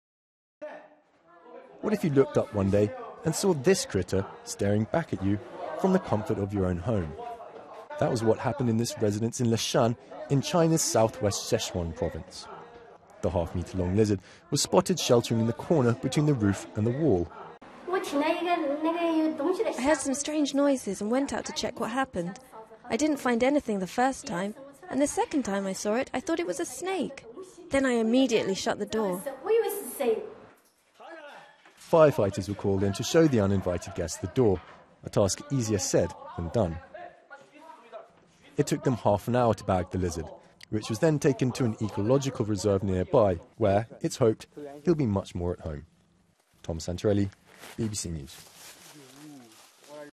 BBC News Firefighters called to catch uninvited lizard in Chinese house
A large lizard has been captured in a house in China's Sichuan province.

It took firefighters 30 minutes to wrangle the creature into a bag so it could be released in a nearby ecological reserve.